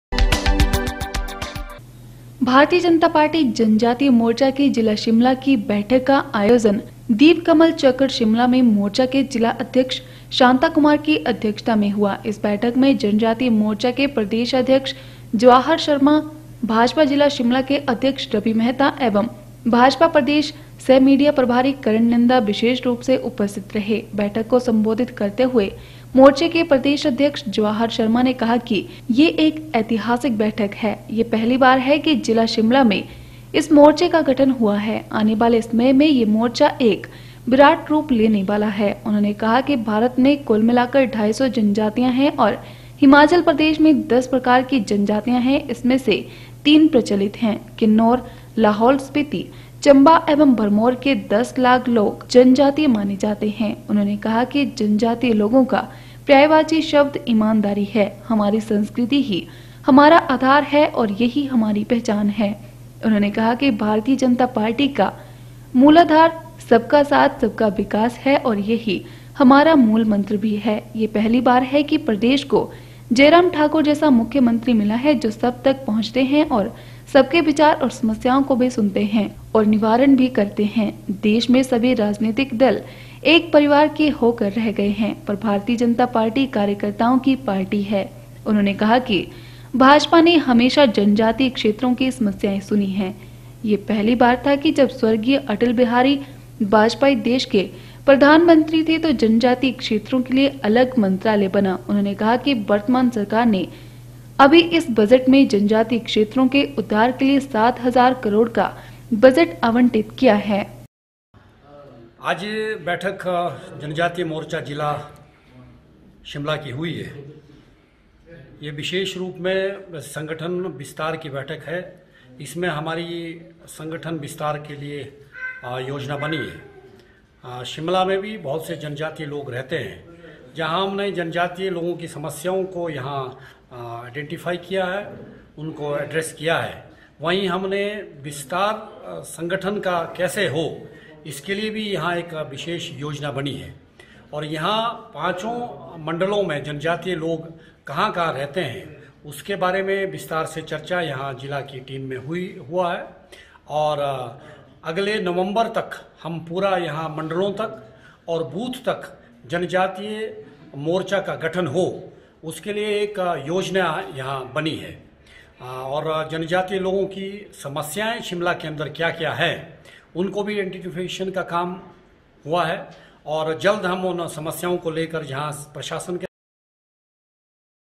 28 OCT 9 दीप कमल चक्कर में मोर्चा के ज़िला अध्यक्ष शांता कुमार की अध्यक्षता किया गया बैठक का आयोजन
भारतीय जनता पार्टी जनजातीय मोर्चा की ज़िला शिमला की बैठक का आयोजन दीप कमल चक्कर शिमला में मोर्चा के ज़िला अध्यक्ष शांता कुमार की अध्यक्षता में हुआ इस बैठक में जनजातीय मोर्चा के प्रदेश अध्यक्ष जवाहर शर्मा भाजपा जिला शिमला के अध्यक्ष रवि मेहता एवं भाजपा प्रदेश सह मीडिया प्रभारी करण नंदा विशेष रूप में उपस्थित रहे।